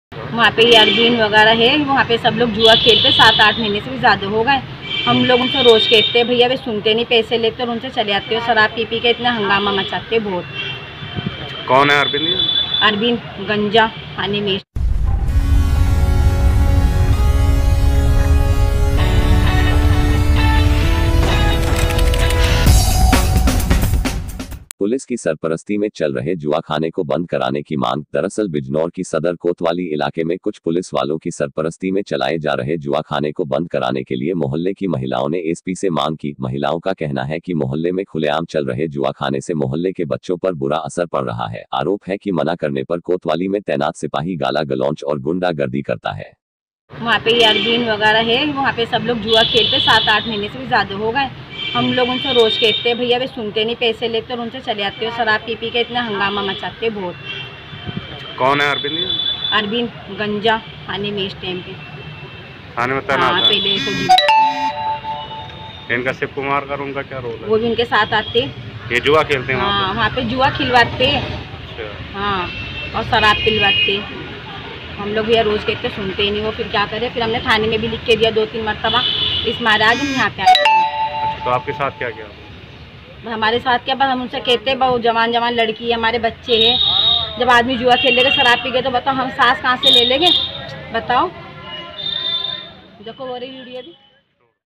पुलिस सरपरस्ती में चल रहा जुआ ख़ाना बन्द कराने की माग
पुलिस की सरपरस्ती में चल रहे जुआ खाने को बन्द कराने की मांग, दरअसल बिजनौर की सदर कोतवाली इलाक़े में कुछ पुलिस वालों की सरपरस्ती में चलाये जा रहे जुआ ख़ाने को बन्द कराने के लिए मोहल्ले की महिलाओं ने एसपी से मांग की, महिलाओं का कहना है कि मोहल्ले में खुलेआम चल रहे जुआ ख़ाने से मोहल्ले के बच्चों पर बुरा असर पड़ रहा है, आरोप है कि मना करने पर कोतवाली में तैनात सिपाही गाला गलौंच और गुण्डा गर्दी करता है